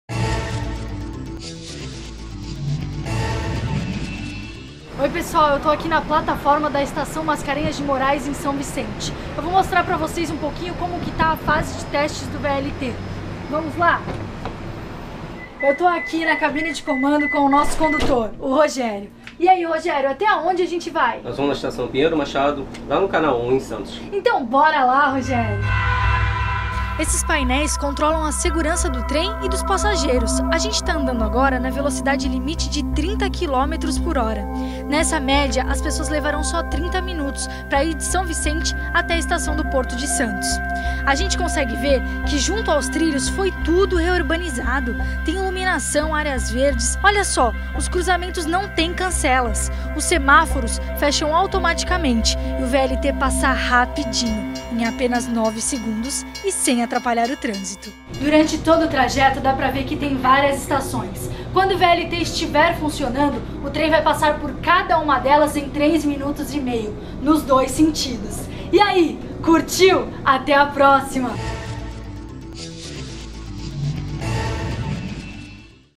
Especial VLT: confira como anda a fase de testes do modal
Nossa repórter Isabella Pajón fez uma viagem da estação Mascarenhas de Moraes, em São Vicente, até a estação Pinheiro Machado, no canal 1, em Santos. Confira, no especial sobre o Veículo Leve sobre Trilhos, mais novidades do novo modal que vai revolucionar o transporte público na região.